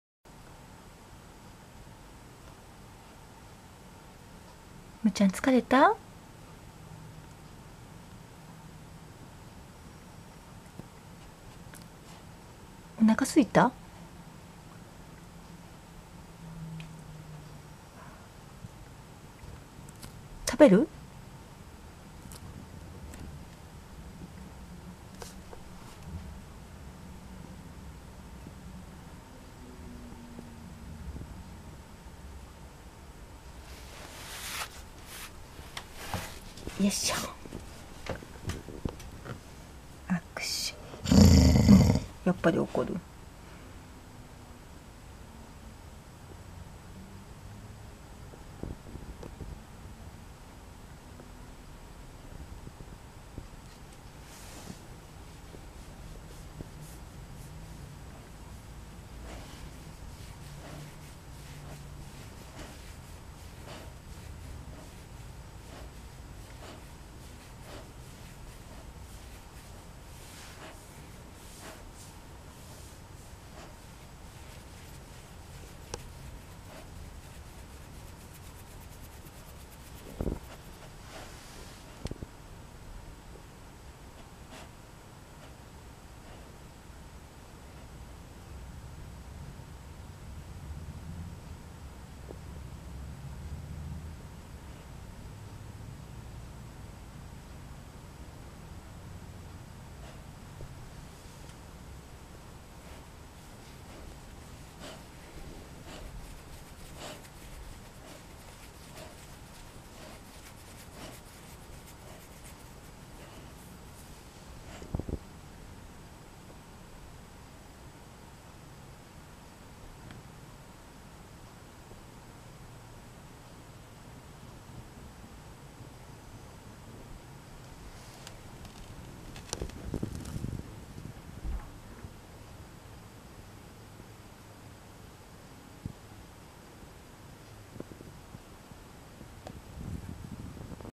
小屋でまったり寛ぐムーちゃん ～癒されます～
この日はムーちゃんと二人きり♡小屋で寛ぐムーちゃんとのんびり時間を過ごしました。